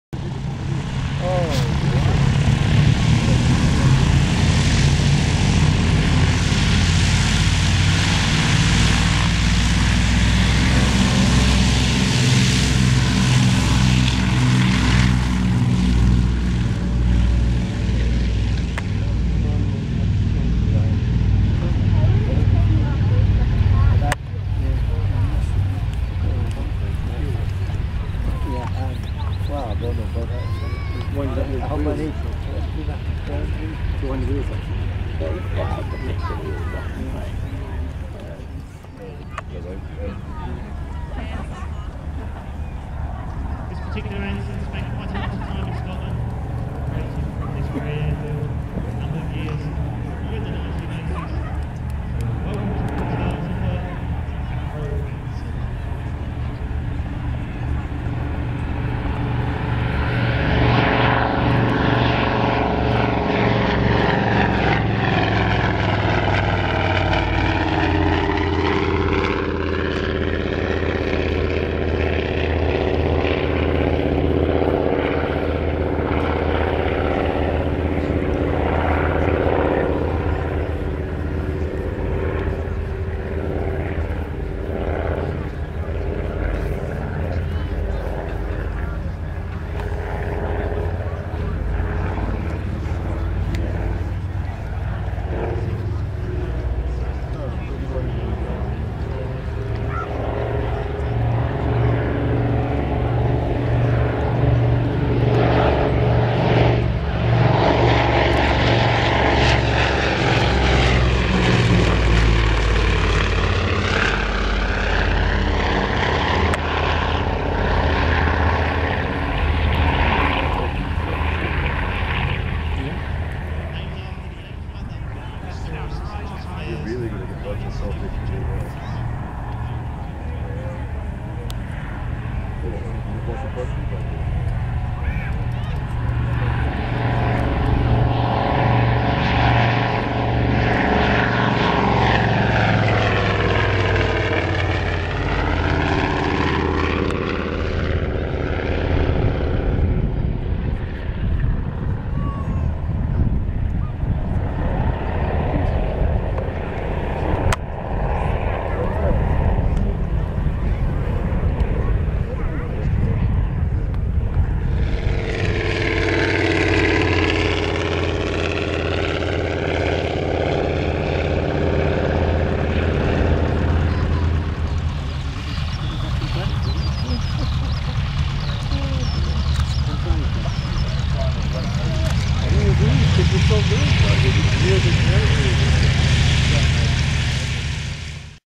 Avro Anson T21 from Air Atlantique Classic Flight displays at Perth Airshow
Avron Anson T21 from Air Atlantique Classic Flight displays at Perth Airshow